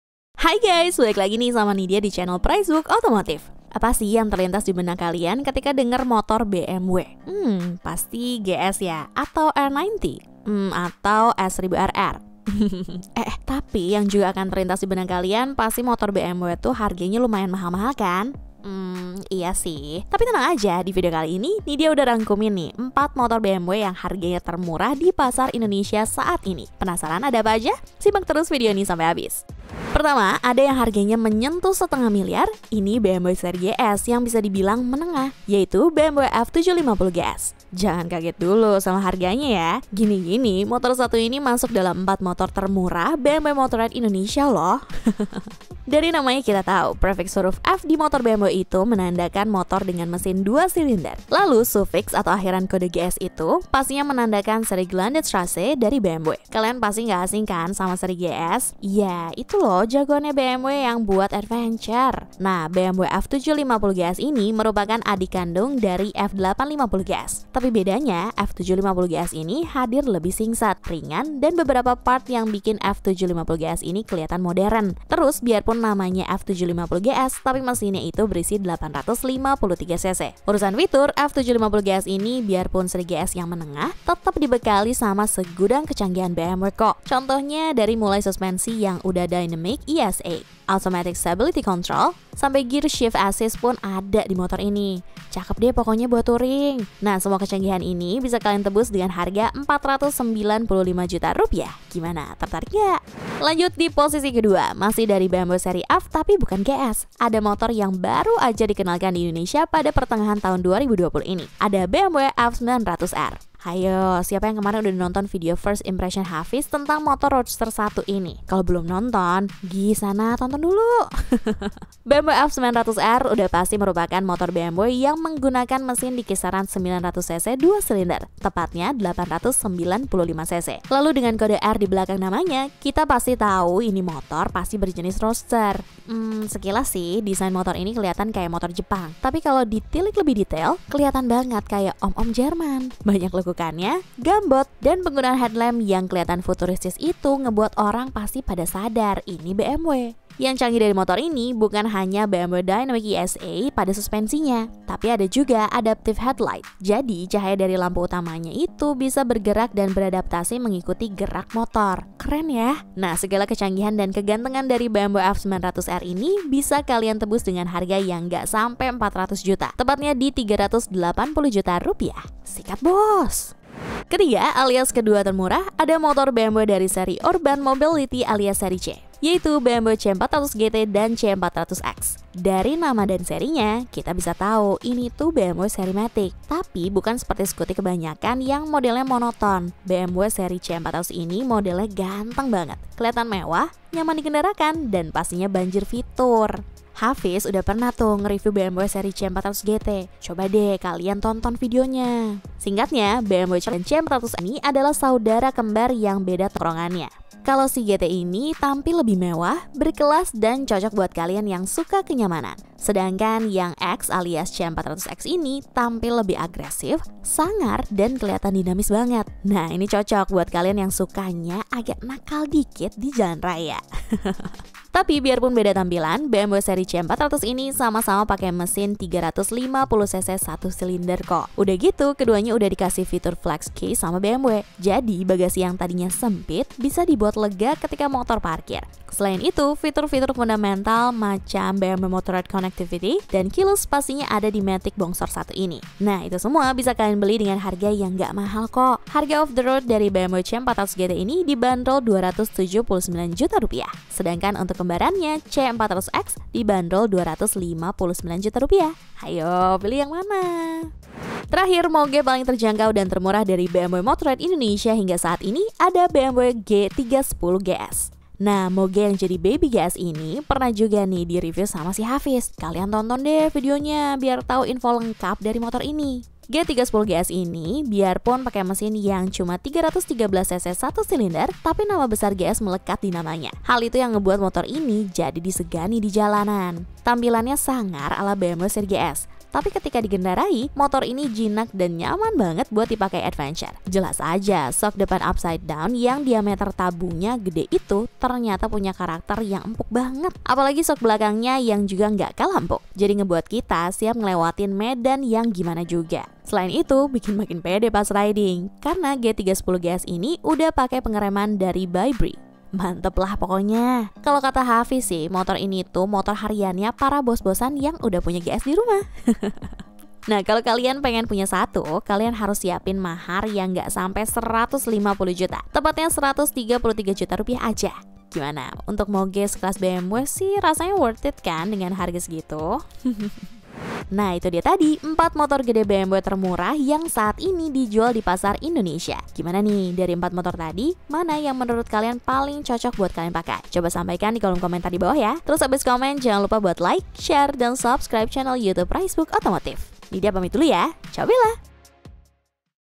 MOGE BMW TERMURAH! Daftar 4 Motor Gede BMW dengan Harga Paling Terjangkau!
Video kali ini, Nidya mau merekomendasikan lima moge BMW termurah yang bisa dibeli saat ini. Kira-kira harga dari moge asal Jerman ini ada yang terjangkau gak ya, daripada penasaran mending kalian simak terus video ini sampai habis!